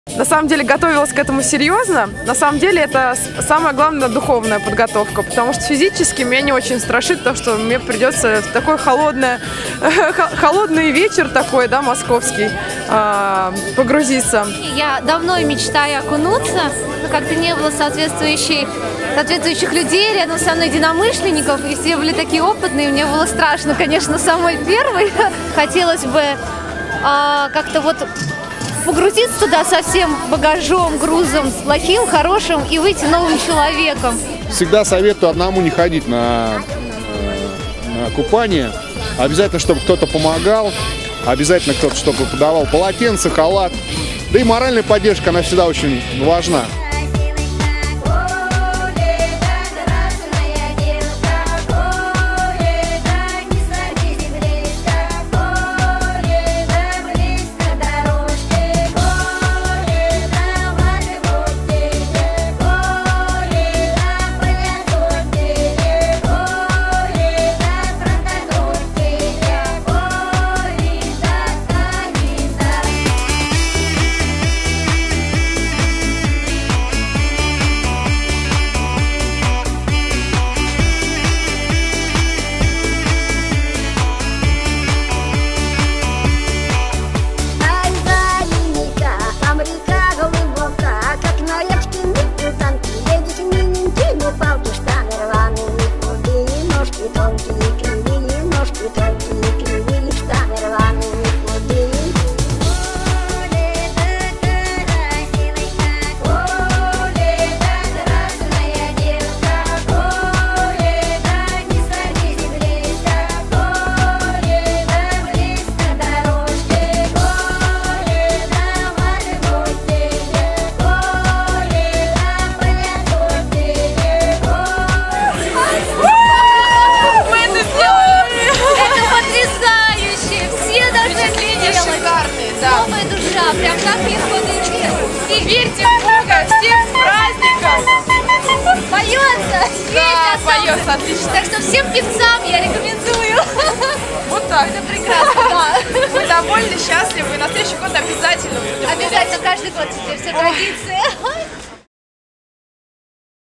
Верующие отмечают Крещение Господне
С вечера пятницы в Москве стоит двадцатиградусный мороз